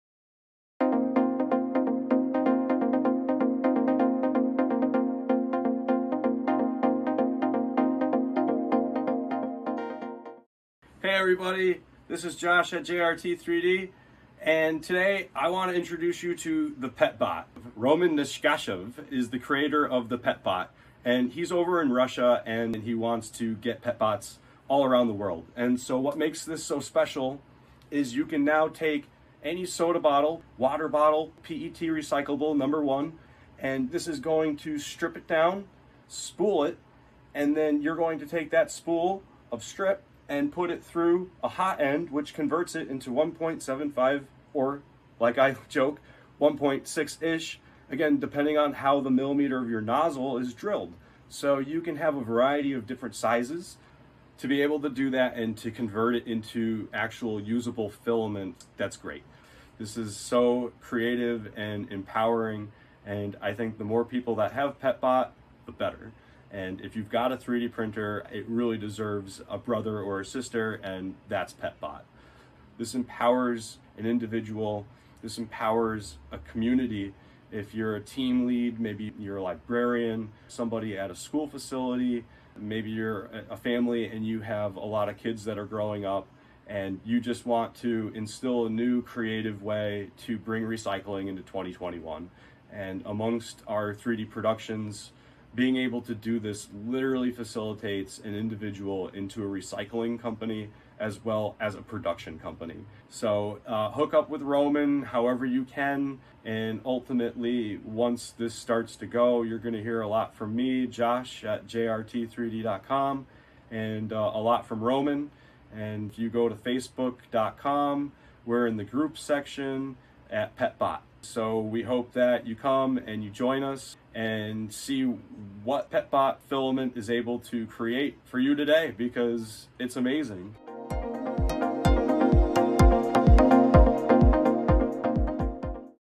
PETBOT - Recycle Soda Bottles into 3D PET Filament
With no more filament needed, PETBOT machine allows you to make filament out of PET Plastic Bottles.

All pics are 3D prints from bottles of different colors and brands, made by us and our customers.

You can seam weld different color filaments together to get multicolored 3d prints (see accumulator holder picture).

Currently we have customers in USA, Austria, Romania, Portugal, and Canada.

Will you be next?

PETBOT - Recycle Soda Bottles into 3D PET Filament - Showcased by Josh @ JRT3D.com